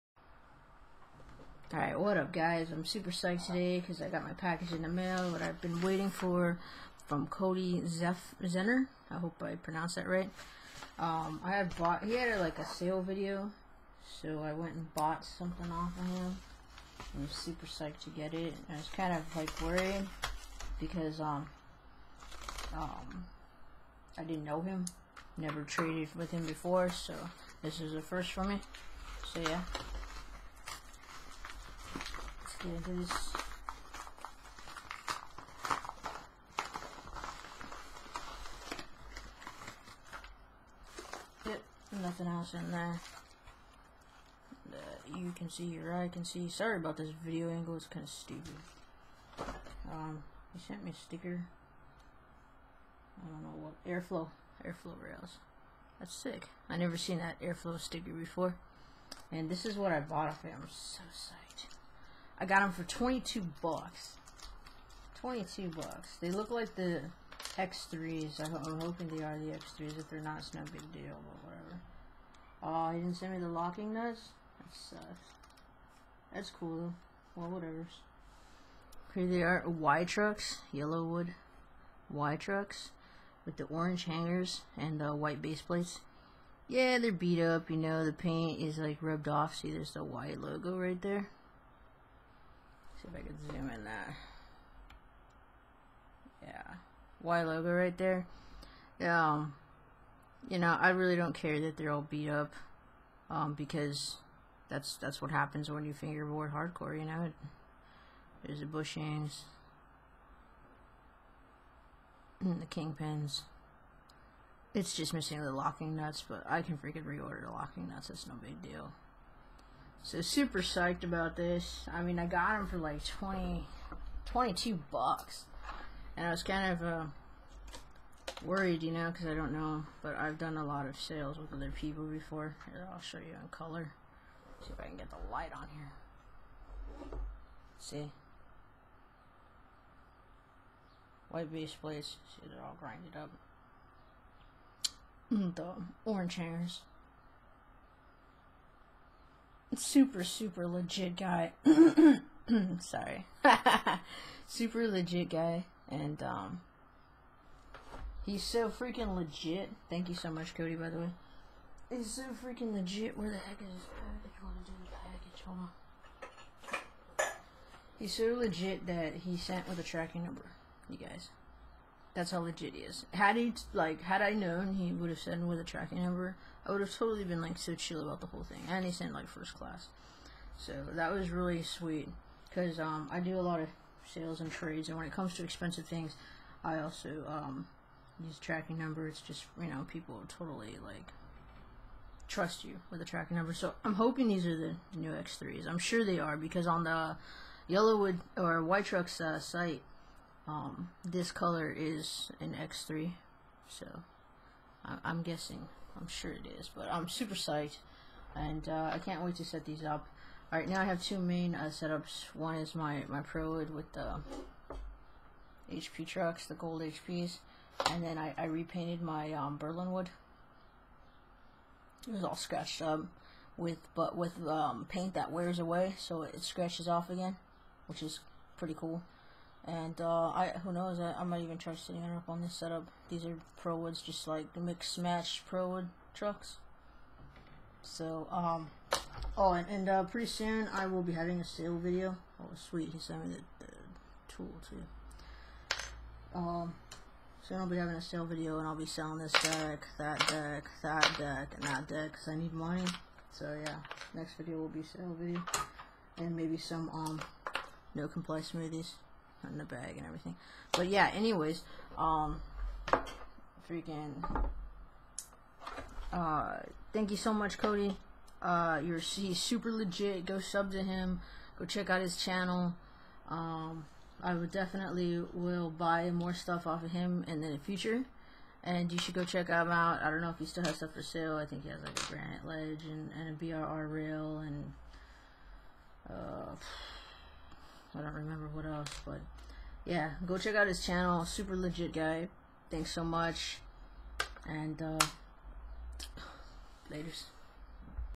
Y trucks X3 unboxing
Unboxing some y trucks from czehn94, he's super legit! Go sub to his channel, and buy something off of him!